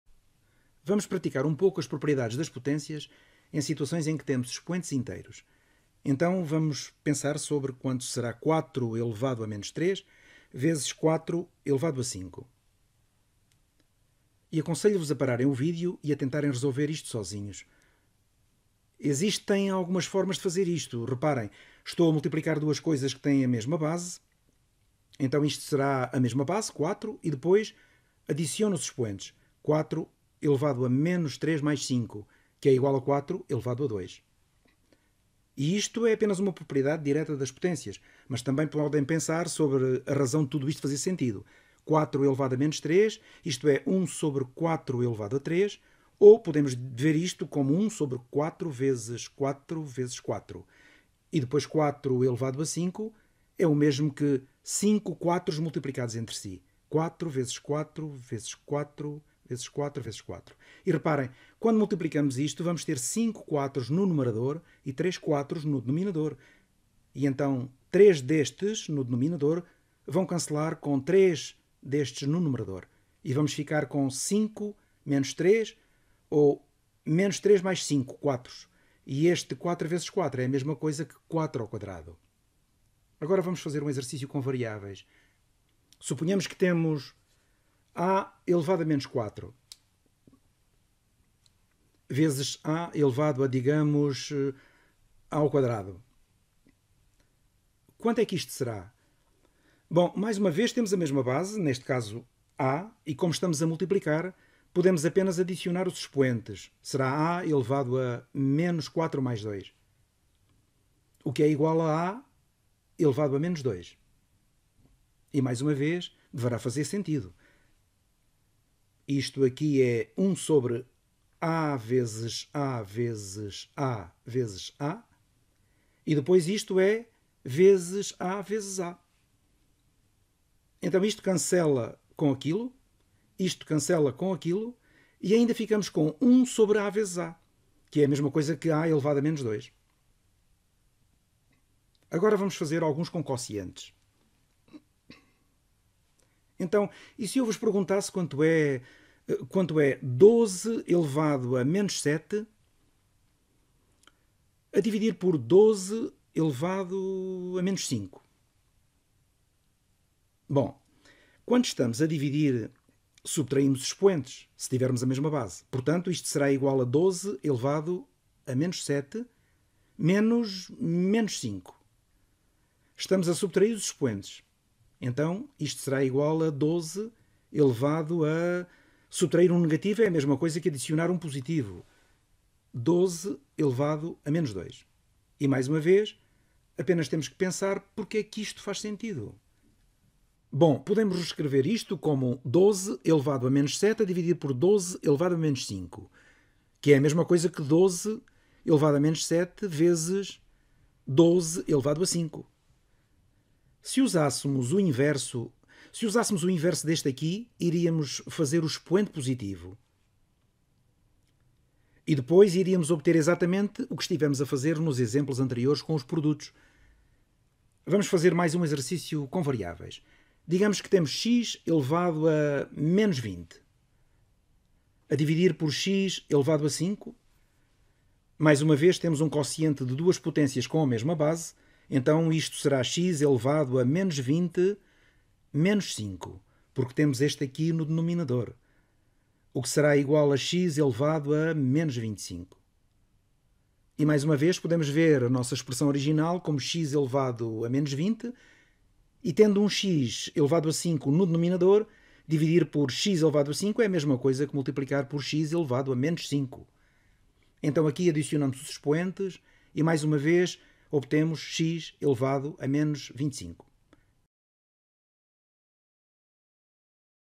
Multiplicação e divisão de potências (expoentes inteiros) - Khan Academy em português (8º ano)
Vídeo original: Multiplying & dividing powers (integer exponents)